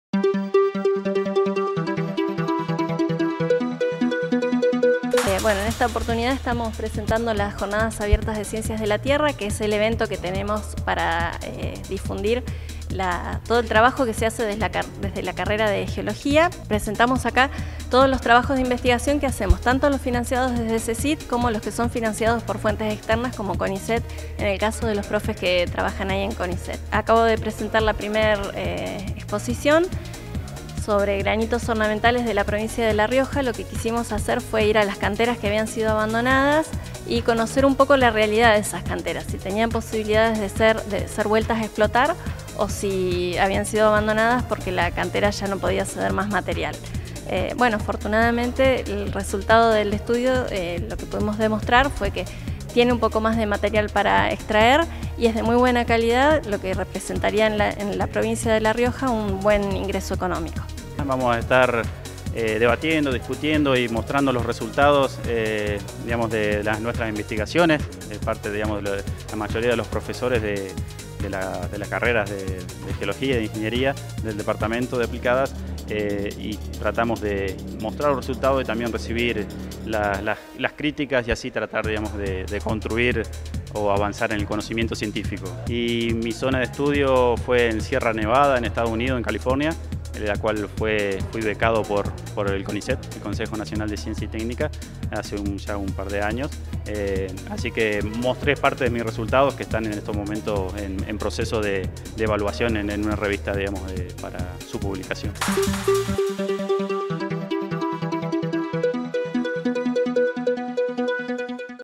U Noticias - Con Jornadas de Ciencias de la Tierra difunden la carrera de geología
Con Jornadas de Ciencias de la Tierra difunden la carrera de geología. Docentes y estudiantes presentaron trabajos de investigación en la materia.